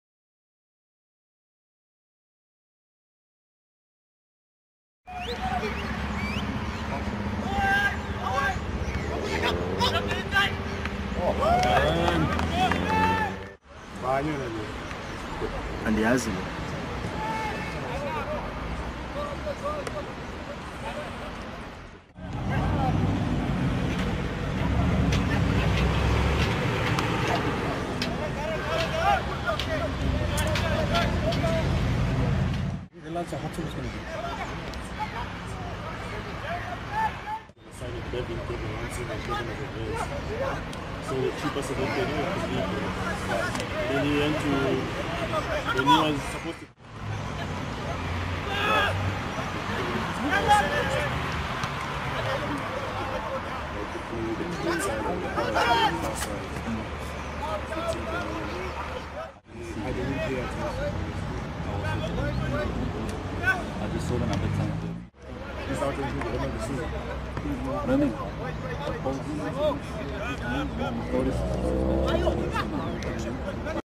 Darren Keet vs Cape Town Spurs
My first match back in 10 months after a Rectus Femoris Evulsion.